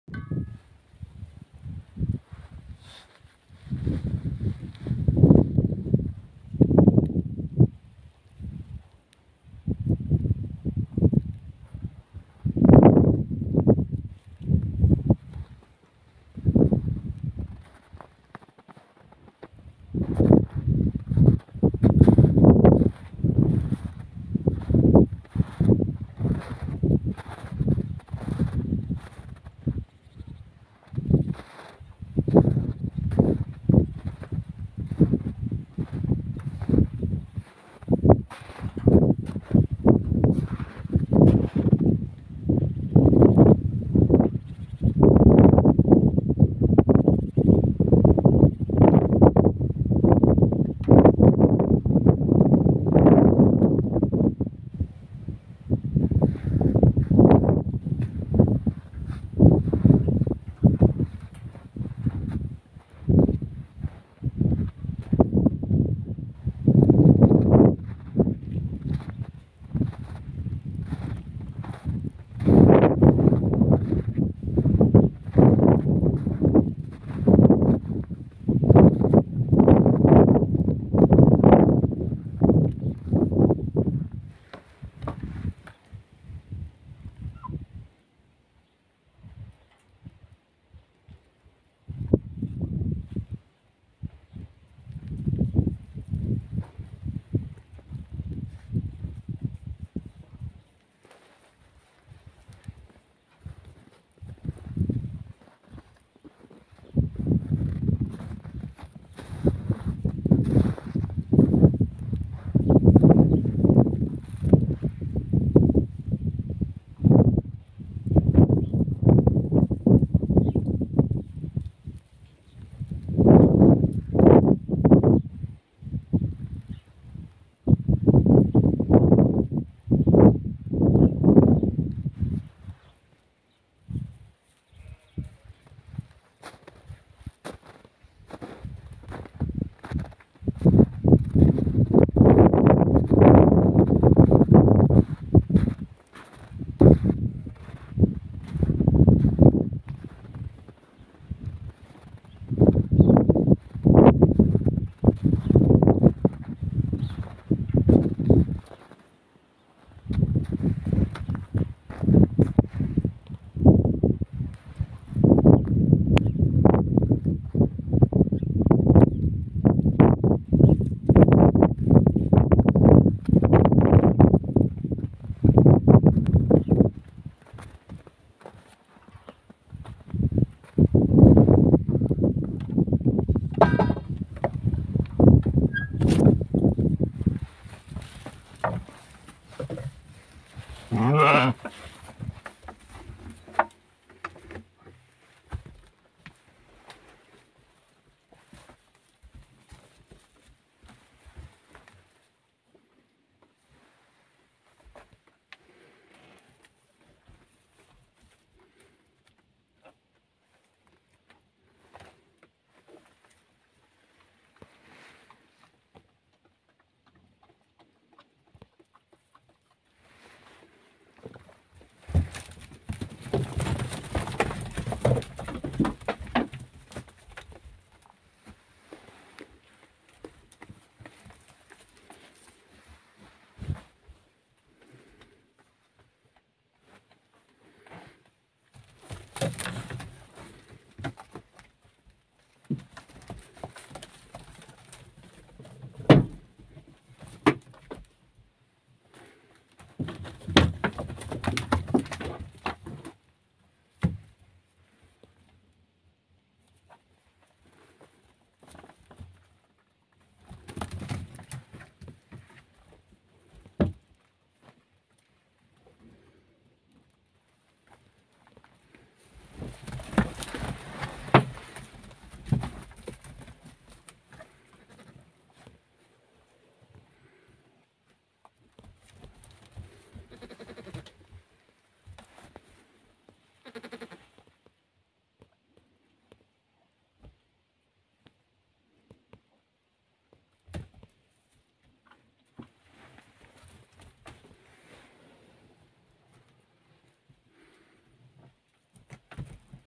Гиссар тұқымдас қой! Қой моншада! Наши овцы в бани! Our sheep in the baths!
Қой моншада! Боран. Гиссар тұқымды қошқар. Ягнята.